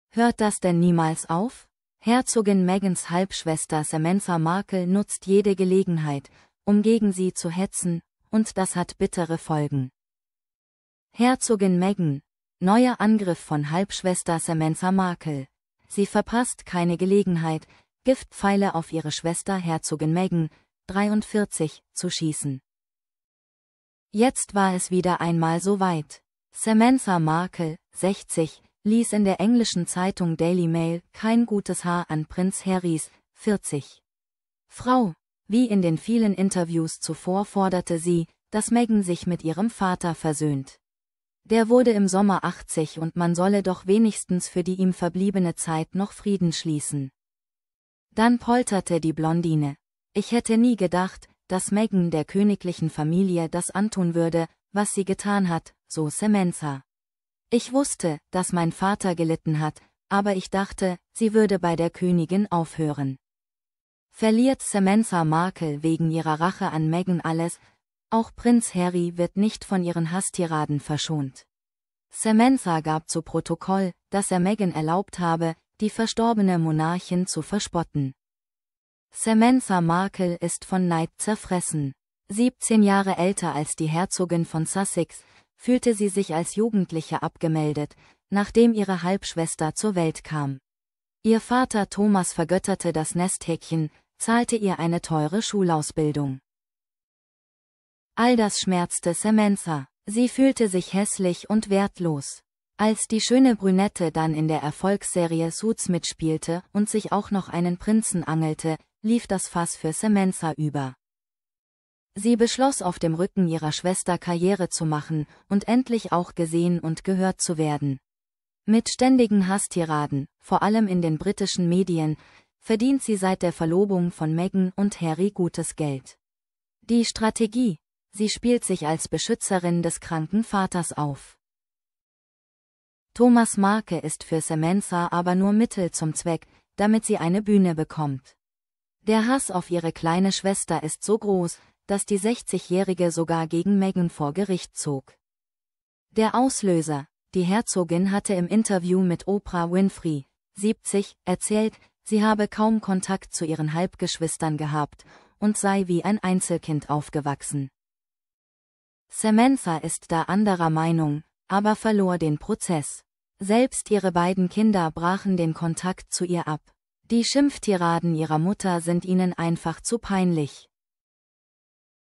Herzogin Meghan: Von Neid zerfressen! Neue Vorwürfe von Halbschwester Samantha
Hört das denn niemals auf? Herzogin Meghans Halbschwester Samantha Markle nutzt jede Gelegenheit, um gegen sie zu hetzen - und das hat bittere Folgen.